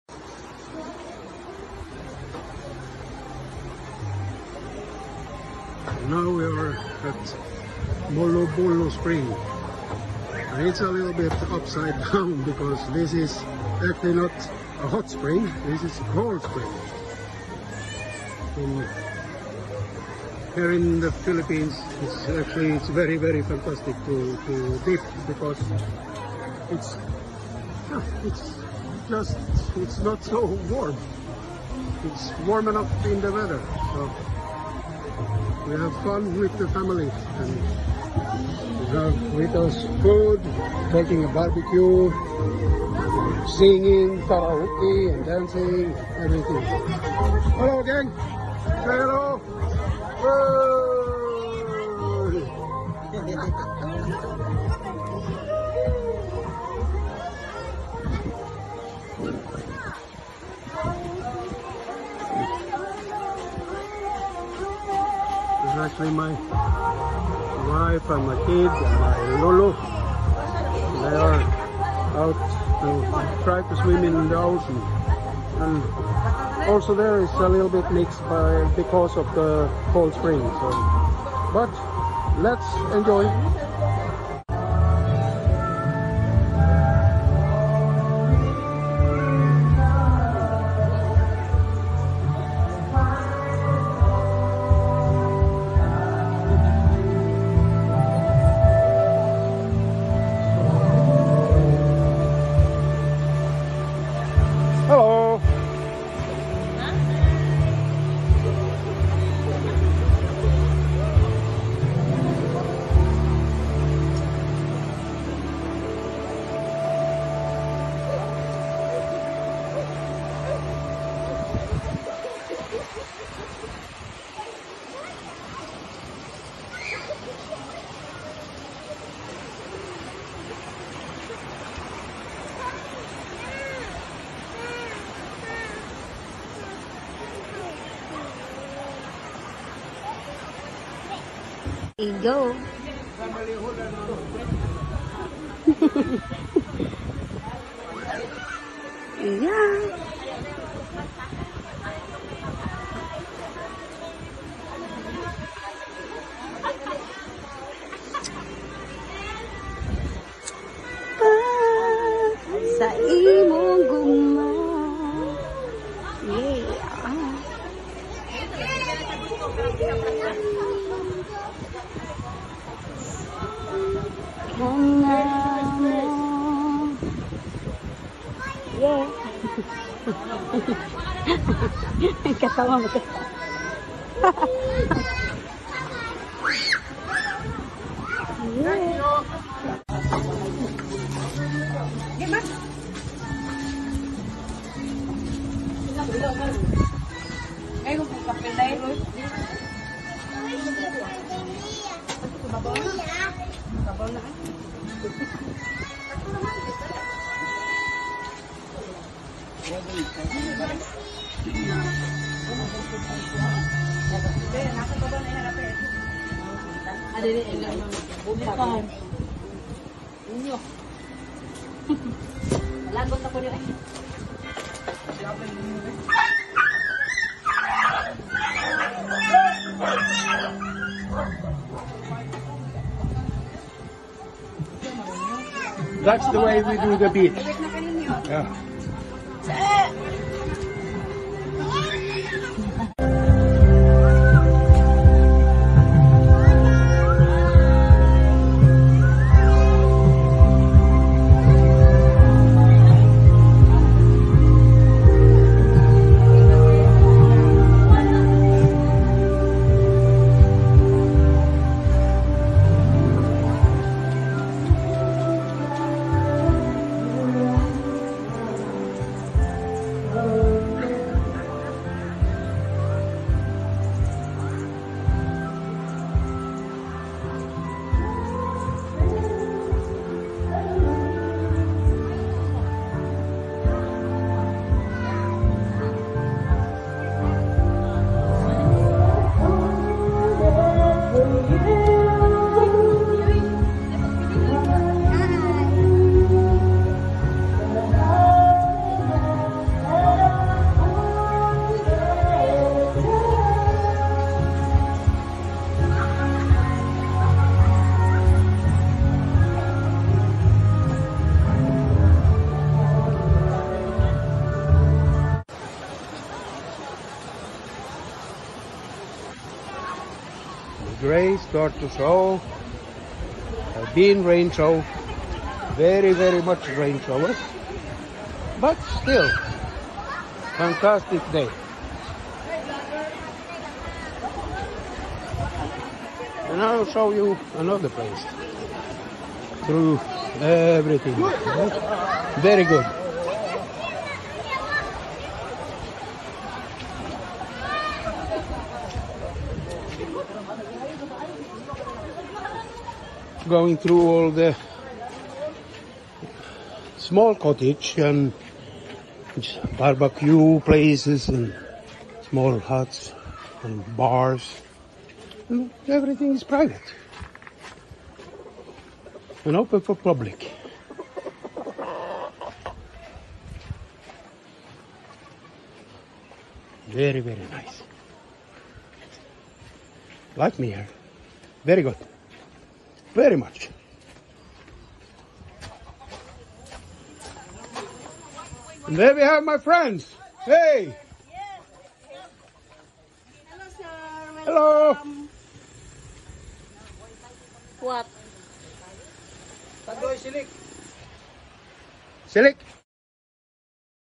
Molobolo Spring
Molobolo Spring is a cold spring, and here it is delicious and refreshing to bathe, it is located right by the sea. If you visit this place, feel free to take a walk in between the small houses and get to know the local residents there better. They have small pubs and welcome overnight guests. Fantastic people